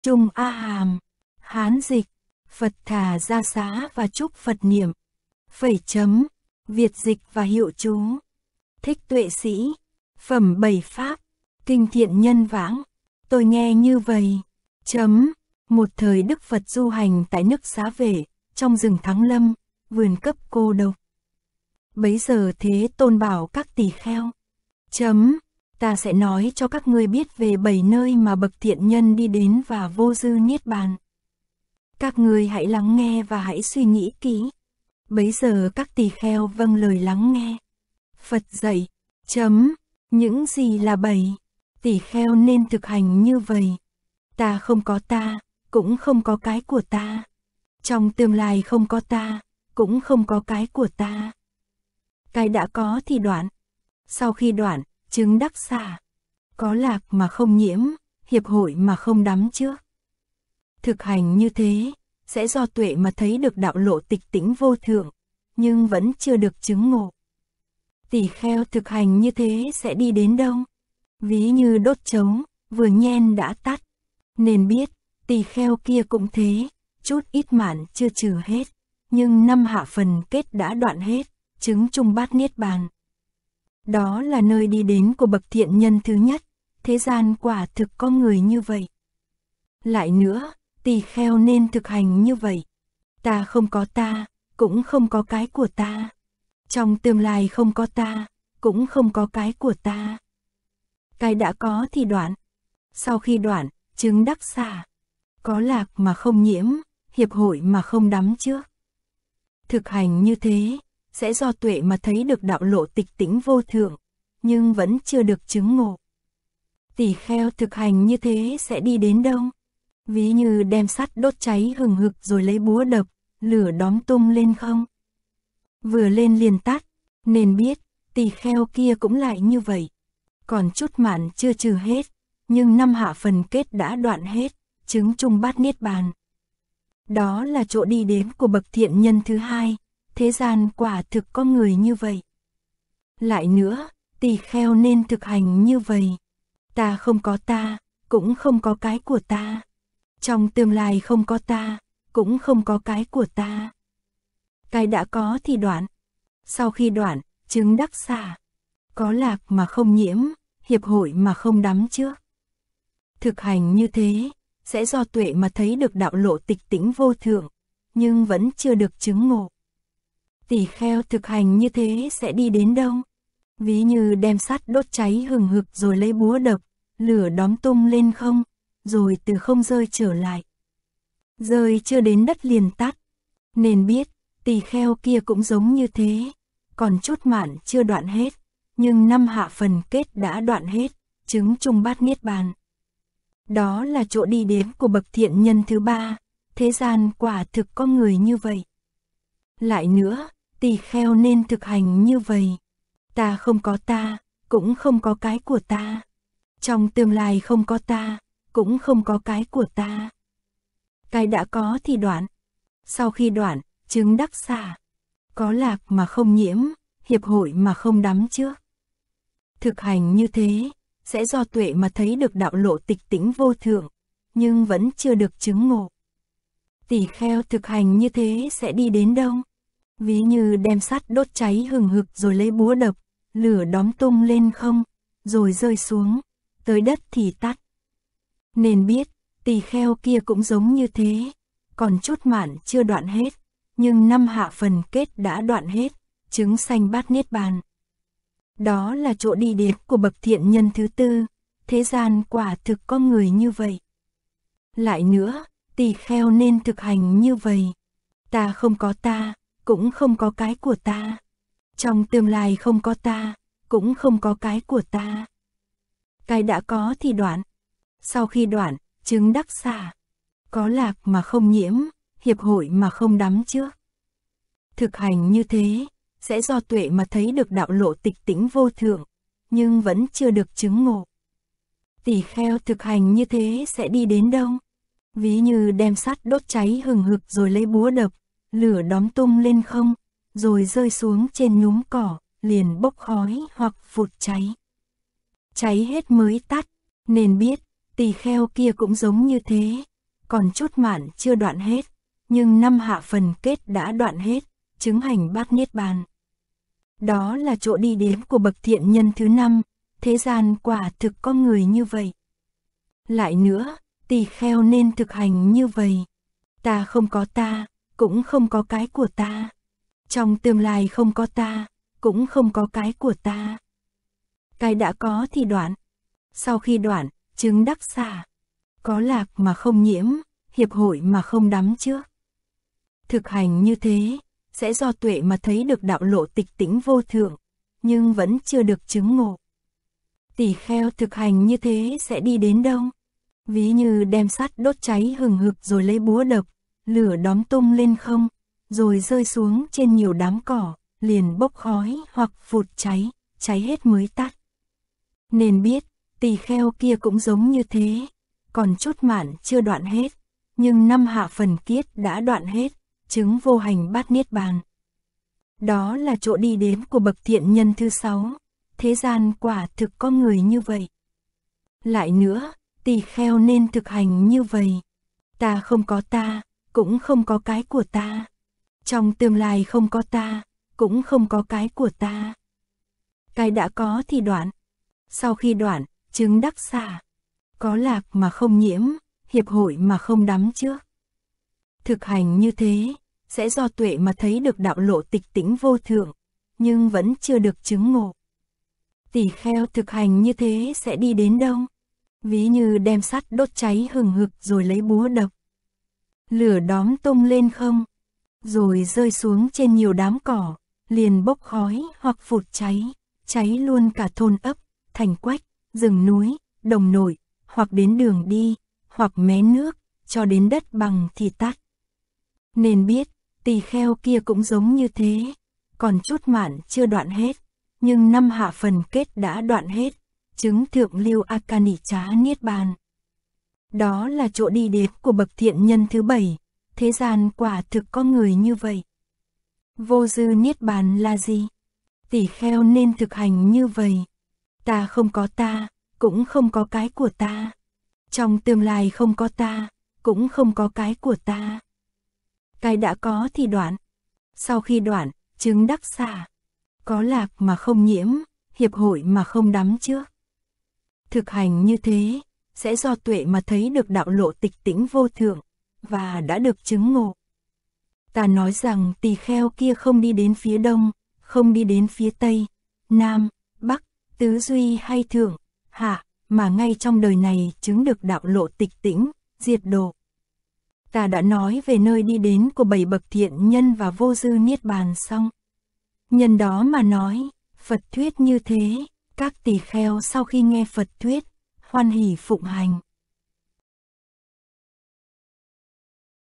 TAHA_006_ Kinh thiện nhân vãng, TrungAham, Thich Tue Si (Aug 2024)
tỳ-kheo nên thực hành như vầy: ‘ta không có ta, cũng không có cáI của ta. Trong tương lai không có ta, cũng không có cáI của ta. CáI đã có thì đoạn’. Sau khi đoạn, chứng đắc xả. Có lạc mà không nhiễm, hiệp hộI mà không đắm trước. Thực hành như thế, sẽ do tuệ mà thấy được đạo lộ tịch tĩnh vô thượng, nhưng vẫn chưa được chứng ngộ. Tỳ-kheo thực hành như thế sẽ đI đến đâu? Ví như đem sắt đốt cháy hừng hực rồI lấy búa đập, lửa đóm tung lên không, rồI rơI xuống, tớI đất thì tắt. Nên biết, tỳ-kheo kia cũng giống như thế, còn chút mạn chưa đoạn hết, nhưng năm hạ phần kết đã đoạn hết, chứng sanh bát-niết-bàn . Đó là chỗ đI đến của bậc thiện nhân thứ tư, thế gian quả thực có ngườI như vậy. “lạI nữa, tỳ-kheo nên thực hành như vầy: ‘ta không có ta, cũng không có cáI của ta. Trong tương lai không có ta, cũng không có cáI của ta. CáI đã có thì đoạn’.

6. Kinh Thiện Nhân Vãng
[1] Bản Hán, bắt đầu quyển 2. Tương đương Pāli: A. VII.52 Purisagati. Đề kinh theo Tống-Nguyên-Minh: “Thiện Nhân Vãng Lai Kinh” Câu-xá 24 (Đại 29, tr.124b, 125a): thiện sĩ thú善 士 趣.
KINH TRUNG A-HÀM
Madhyama Agama
Hán dịch: Phật Đà Da Xá và Trúc Phật Niệm
Việt dịch và hiệu chú: Thích Tuệ Sỹ
Sài gòn 2002
 1. Phẩm bảy pháp
 6. Kinh thiện nhân vãng

For walking on treadmill and let read out loud
Xem trong khi kinh hành trên máy chạy bộ và đọc theo (như tụng kinh)

vn On Telegram